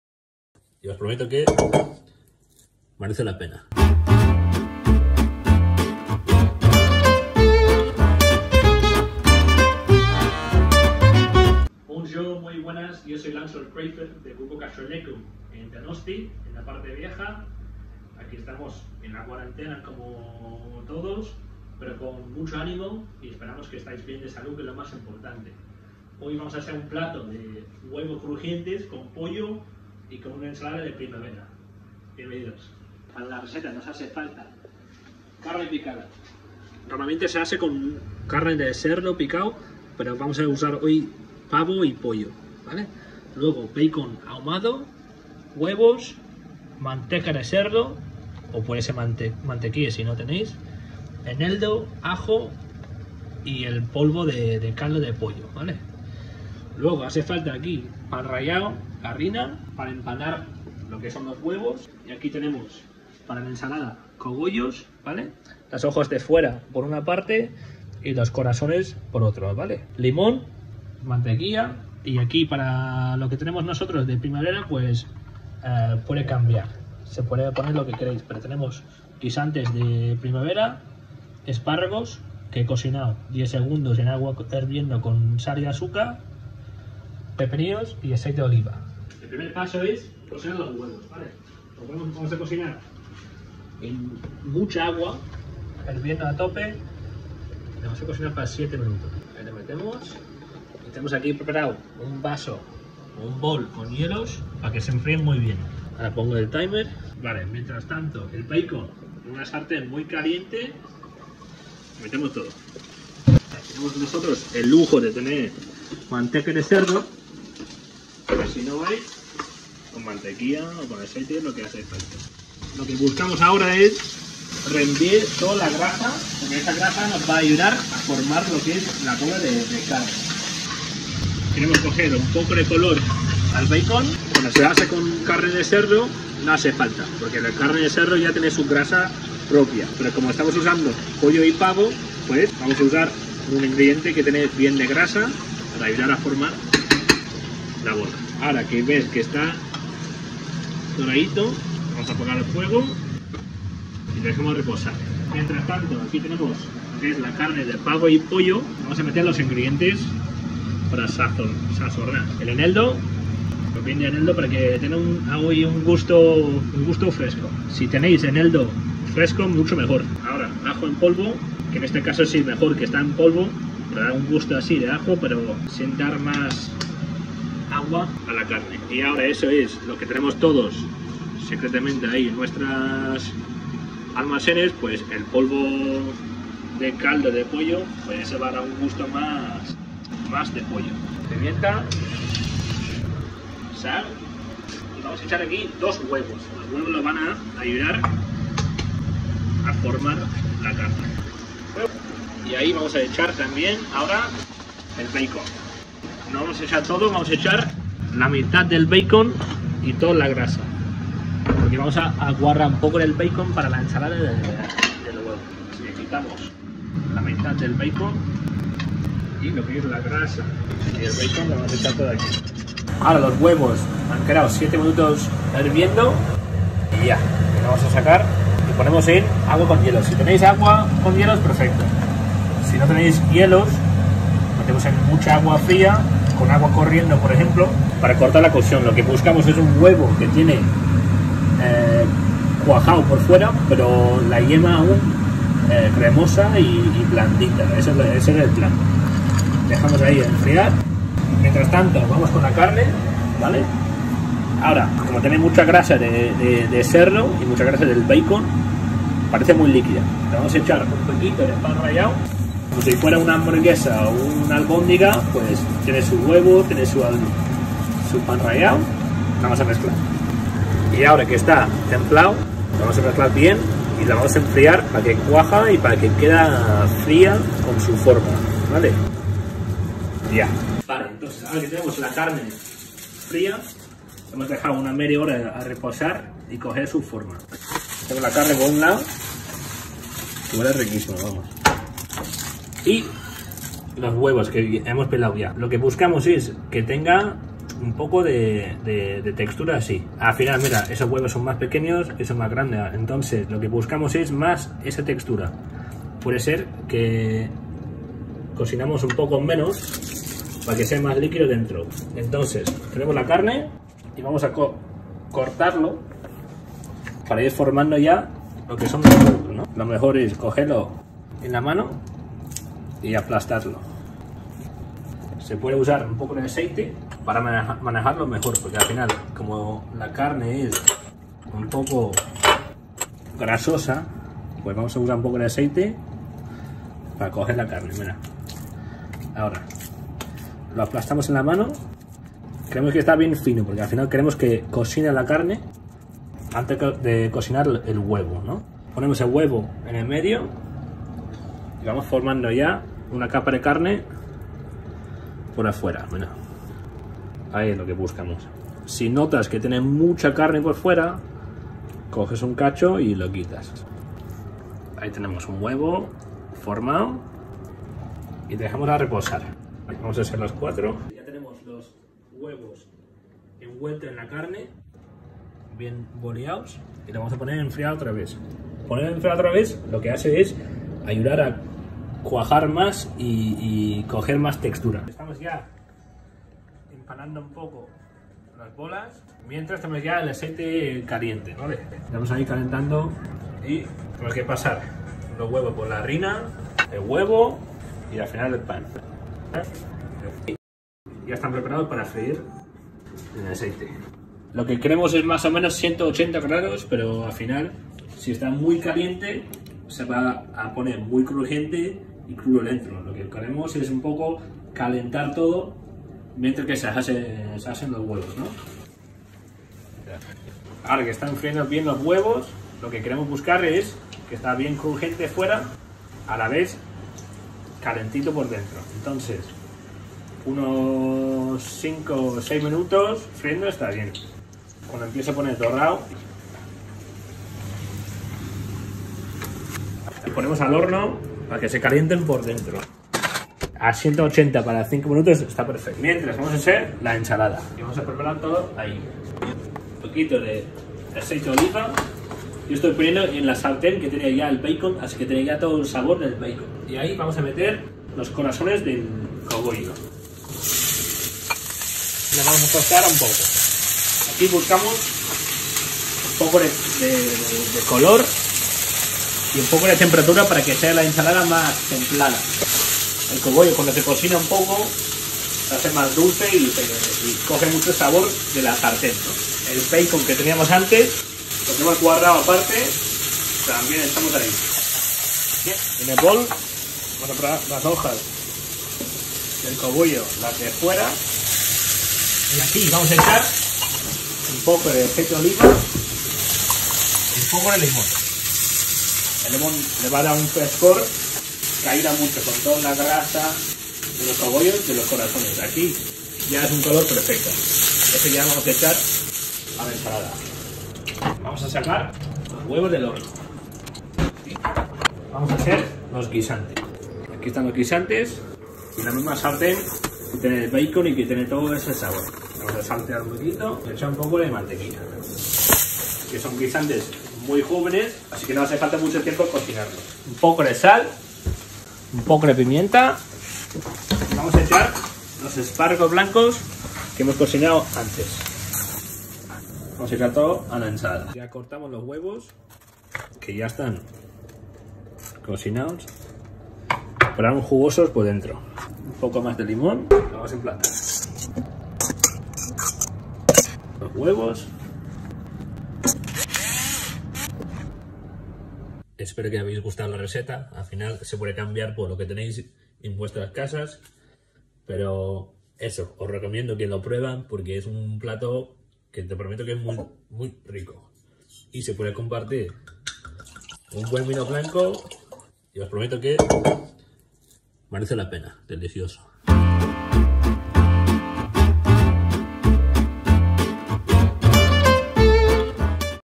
"CONFINÉS" / LES OEUFS CROUSTILLANTS DE LANCELOT
Pour ne pas que vous passiez le "Confinement #1" seuls on a appelé quelques amis chefs et ils ont répondu présents pour ce projet d'une recette par jour.

Voici Lancelot Crafer, chef du restaurant Txalupa à Saint-Sebastien.